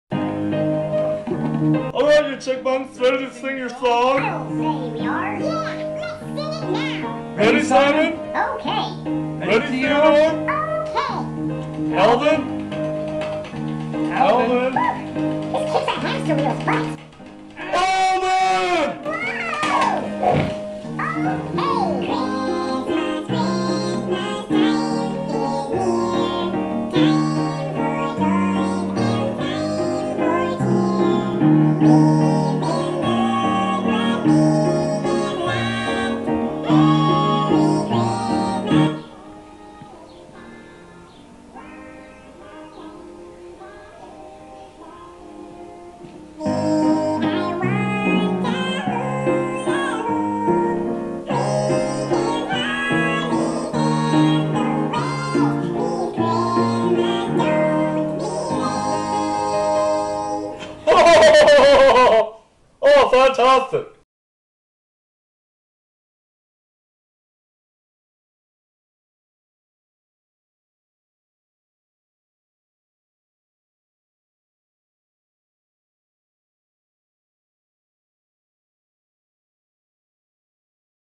Billy Cocktail and The Chipmunks: SE 02 - [Movie Parody]
Youtube exclusive weekly show, Making Drinks with Billy Cocktail. In this special edition Billy hooks up with Alvin, Simon, and Theodore and recreate this wonderful classic song you'll know when you hear it. Enjoy!

If you would like to send mail to Billy Cocktail and have your letter read on the air then please do so at